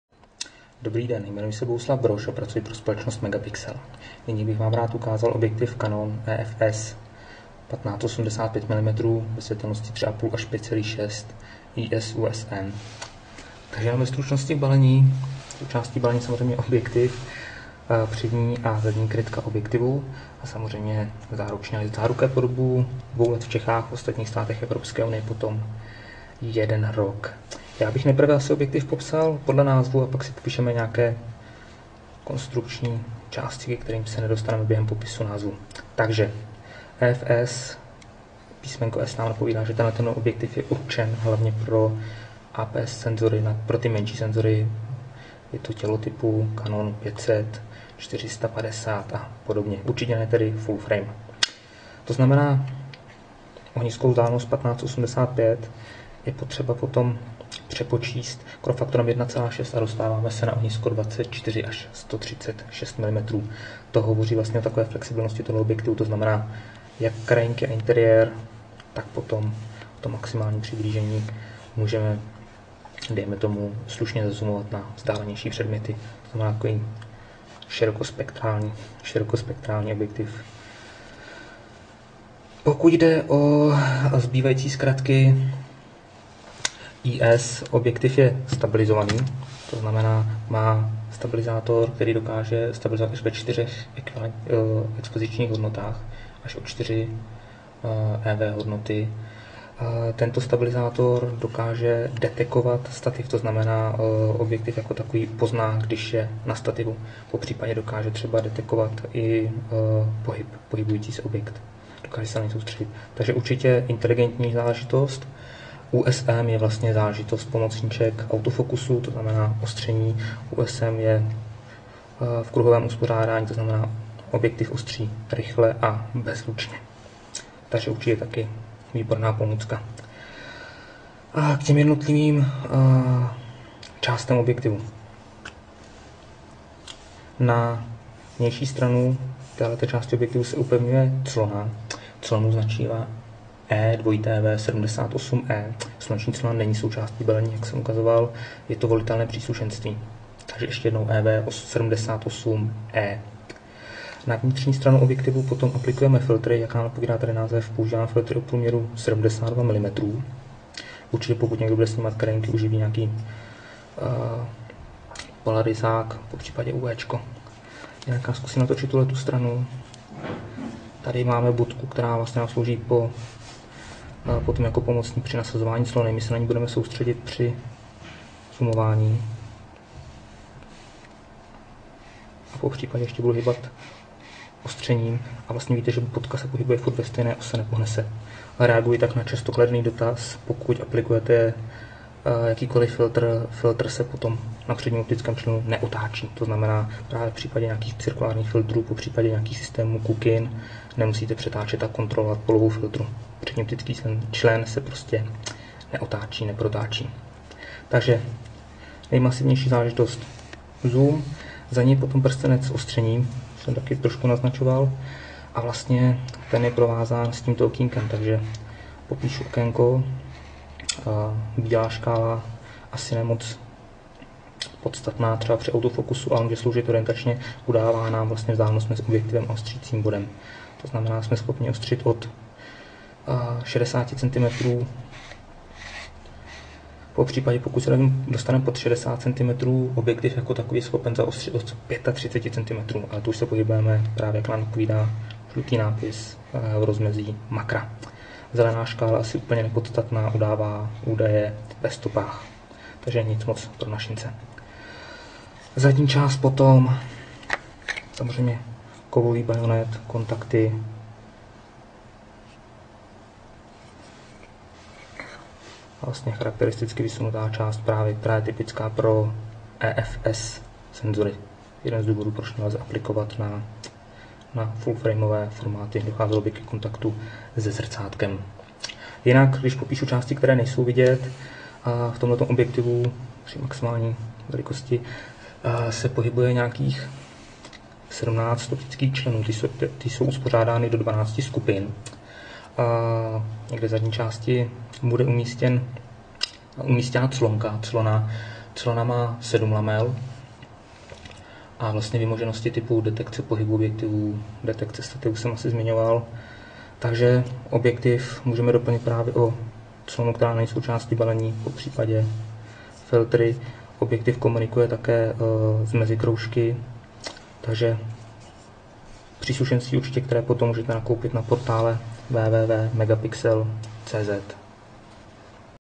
Canon EF-S 15-85mm F/3,5-5,6 IS USM recenze
Představení objektivu pro zrcadlovky Canon EOS.
Veškeré informace o technických parametrech, ceně a dalším příslušenství naleznete na